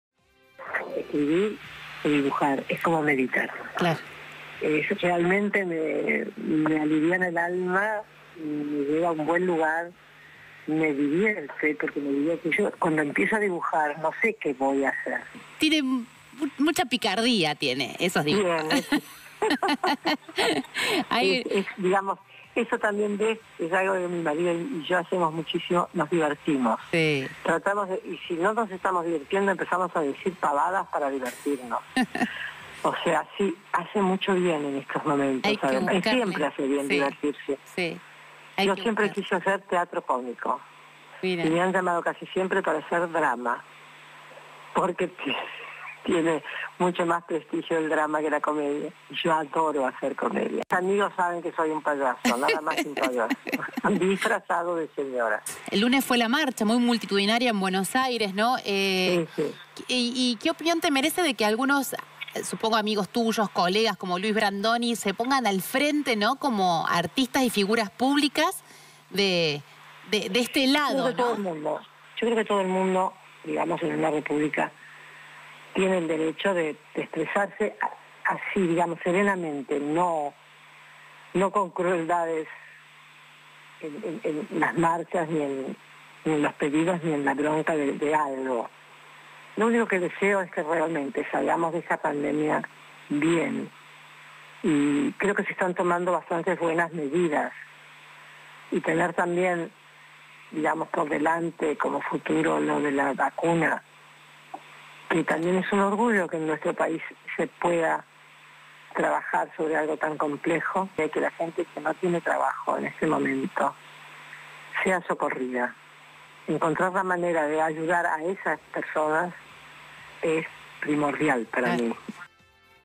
Norma Aleandro: "Escribir y pintar es como meditar"
Norma Aleandro  habló con El Doce sobre su libro "Confieso que pinto". La mayor actriz de la argentina que ha sido protagonista de clásicos del cine argentino como La historia oficial o Cien veces no debo o El hijo de la novia.
Sin embargo se muestra al público con una faceta diferente a la de la actriz. Si bien, como ella misma admite, siempre escribió y dibujó, lo hacía para sí misma y para sus amigos, por gusto, por el placer de interpretar otro lenguaje.